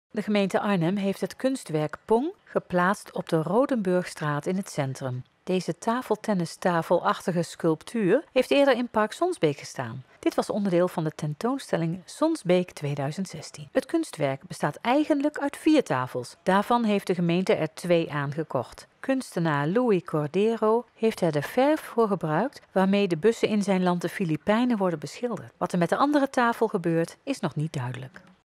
Pingpongtafel in de Rodenburgstraat
De gemeente heeft een kunstwerk ‘Pong’ geplaatst aan de Rodenburgstraat. Het kunstwerk is afkomstig van de Sonsbeektentoonstelling 2016. Het is aangekocht door de gemeente na de beeldententoonstelling.

De kleurrijke pingpongtafel ‘Pong’ is gemaakt door de Filipijnse kunstenaar Louie Cordero en bestaat oorspronkelijk uit vier vrolijk gekleurde pingpongtafels waarvan de gemeente er twee aan heeft gekocht. Vanaf de start van SONSBEEK ‘16 is Arnhem gecharmeerd van de kleurrijke, ongewoon gevormde pingpongtafels van Louie Cordero.

Het zijn publiekslievelingen die door jong en oud gretig gebruikt worden. Cordero haalt een stukje van het kleurrijke en vrolijke straatbeeld van Manilla naar Arnhem. Voor de prints gebruikt hij dezelfde autolak die de stadsbussen (Jeepney’s) in Manilla sieren. Het is daar een sport om de bussen zo origineel mogelijk te beschilderen.